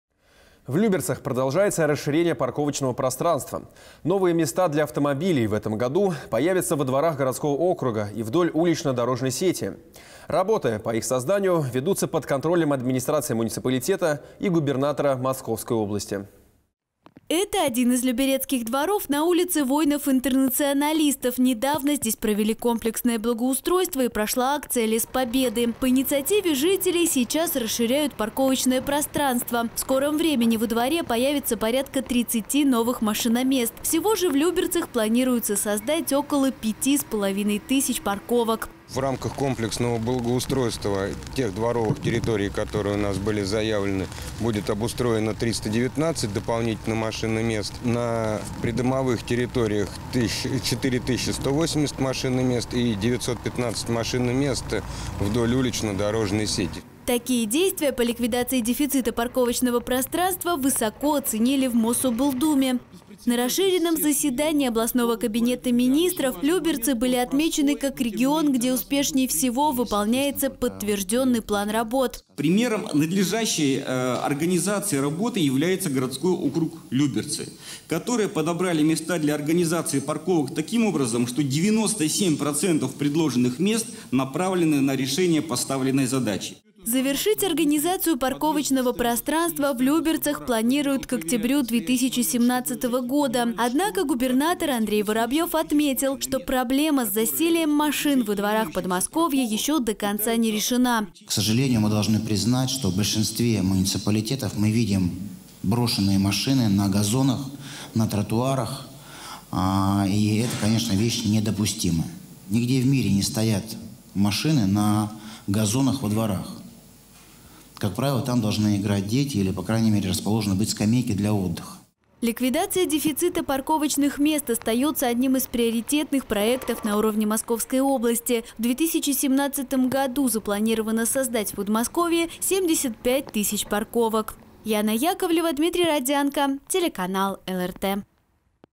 В Люберцах начали строить новое парковочное пространство
В Люберцах продолжается расширение парковочного пространства.  Новые машиноместа в этом году появятся во дворах городского округа и вдоль улично-дорожной сети. Работы по их созданию ведутся  под контролем администрации муниципалитета и губернатора Московской области.

Это один из люберецких дворов на улице Воинов-интернационалистов. Недавно здесь провели комплексное благоустройство и прошла акция «Лес Победы». По инициативе жителей сейчас расширяют парковочное пространство. В скором времени во дворе появится порядка 30 новых машиномест. Всего же в Люберцах планируется создать около пяти с половиной тысяч парковок.

Александр Титов, начальник управления дорожного хозяйства администрации Люберецкого района:

«В рамках комплексного благоустройства тех дворовых территорий, которые у нас были заявлены, будет обустроено319 дополнительных машиномест. На придомовых территориях 4 180 машиномест и 915 машиномест вдоль улично-дорожной сети».

Такие действия по ликвидации дефицита парковочного пространства высоко оценили в Мособлдуме. На расширенном заседании областного кабинета министров Люберцы были отмечены, как регион, где  успешней всего выполняется подтверждённый план работ.

Игорь Тресков, министр транспорта и дорожной инфраструктуры Московской области:

«Примером надлежащей организации работы является городской округ Люберцы, там подобрали места для организации парковок таким образом, что 97 % предложенных мест направлены на решение поставленной задачи».

Завершить организацию парковочного пространства в Люберцах планируют к октябрю 2017 года. Однако губернатор Андрей Воробьев отметил, что проблема с засильем машин во дворах Подмосковья еще до конца не решена.

Андрей Воробьев, губернатор Московской области:

«К сожалению, мы должны признать, что в большинстве муниципалитетов мы видим брошенные машины на газонах, на тротуарах. И это, конечно, вещь недопустимая. Нигде в мире не стоят машины на газонах во дворах. Как правило, там должны играть дети или, по крайней мере, расположены быть скамейки для отдыха».

Ликвидация дефицита парковочных мест остается одним из приоритетных проектов на уровне Московской области. В 2017 году запланировано создать в Подмосковье 75 тысяч парковок.

Яна Яковлева, Дмитрий Родзянко